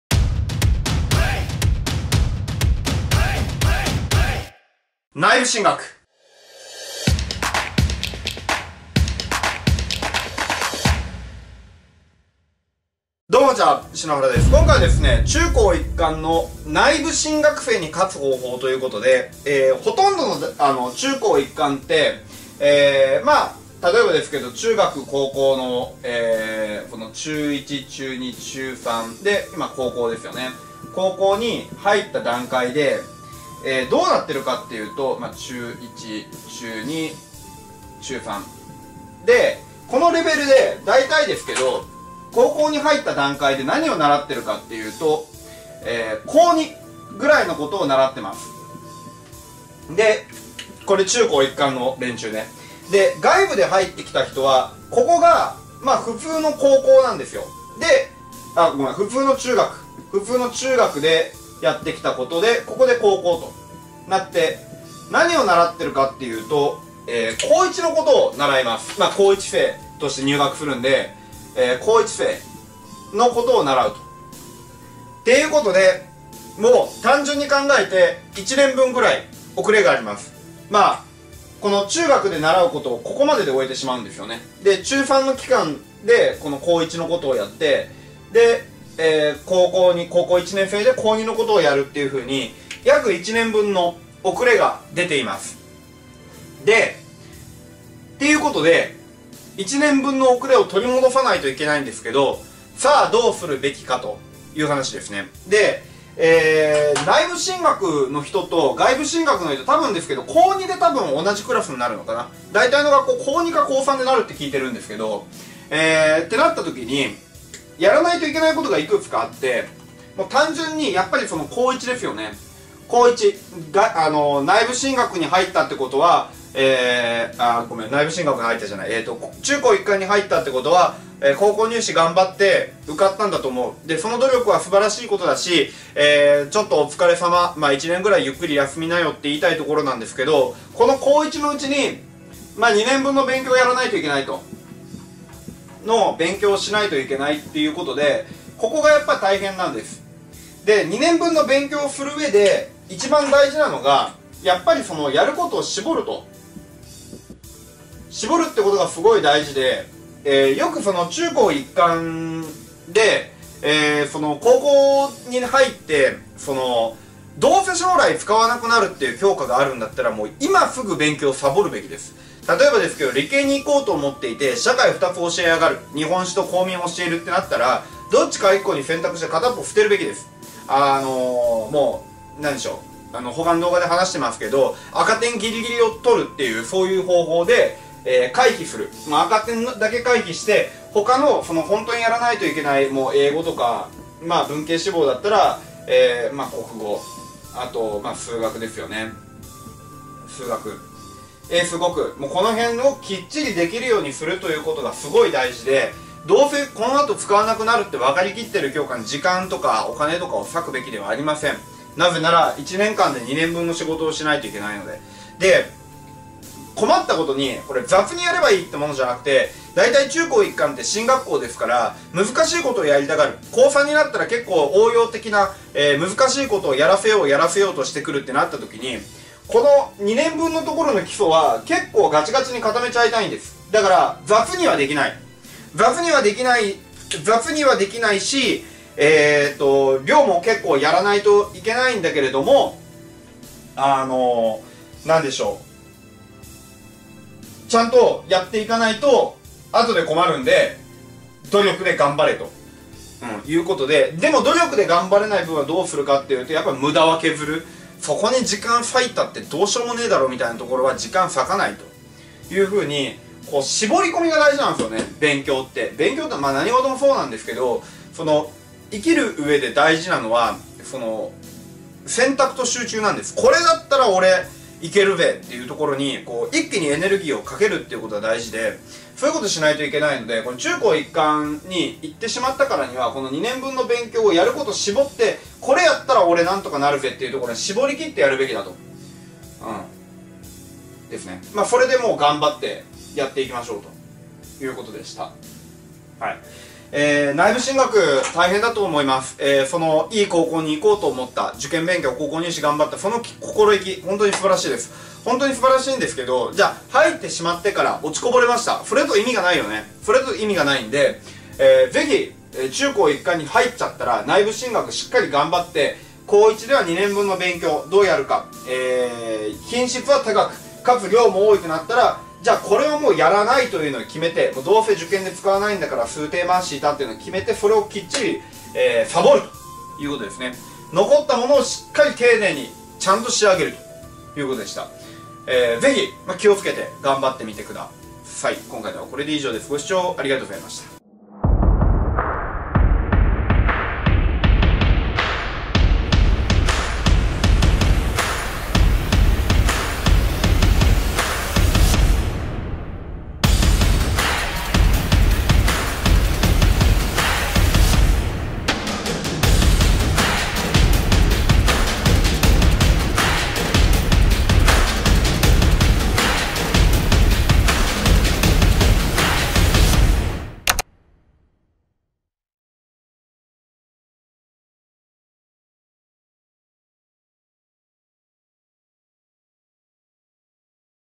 中高一貫で「内部進学」の連中に追いつき、追い越す方法～全国模試1位の勉強法【篠原好】
【動画の説明】
中高一貫で「内部進学」の連中に追いつき、追い越す方法～京大模試全国一位の勉強法【篠原好】

【その他】
BGM...KOU-G様
SE. Bastet tail様